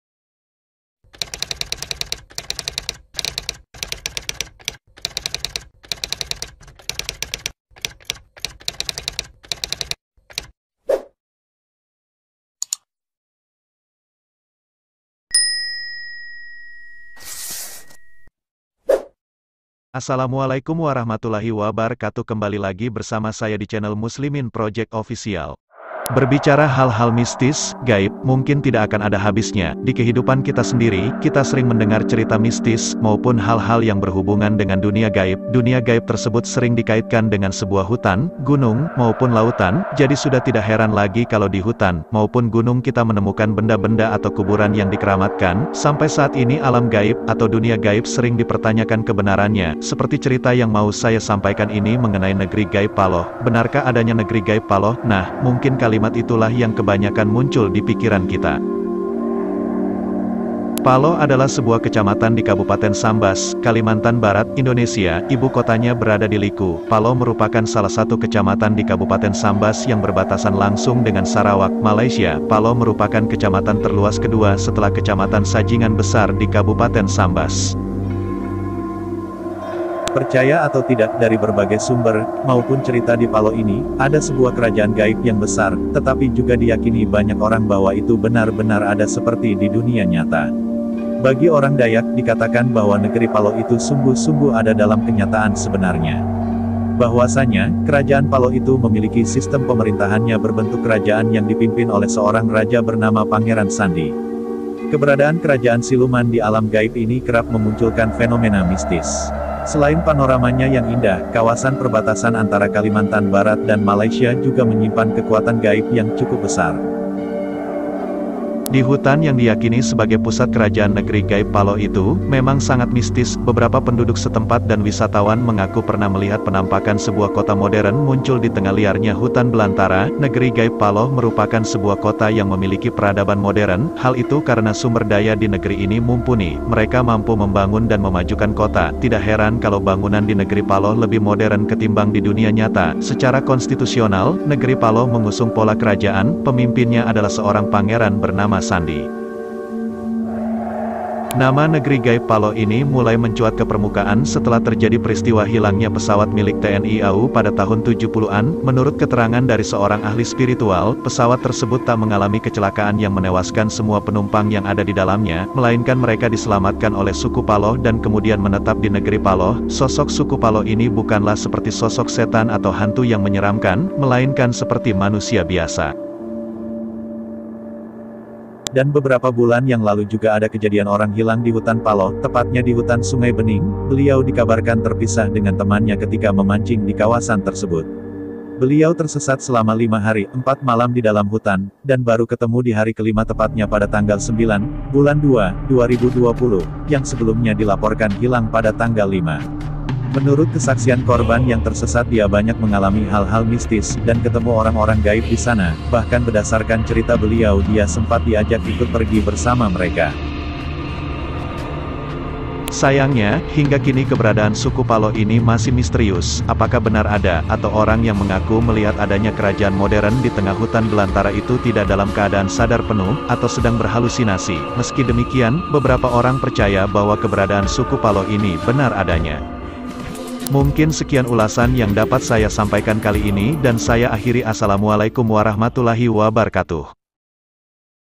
BENARKAH NEGRI GAIB PALOH ITU ADA?
dunia gaib memanglah suatu pembahasan yang menarik untuk kita simak, seakan kehidupan kita tak lepas dari hal mistis, maupun gaib.
 bahkan disuatu daerah tertentu menyimpan kisah mistis, gaib tersendiri.
seperti pembahasan kita kali ini tentang negri gaib paloh.

allahualam, percaya tidaknya tergantung diri masing masing...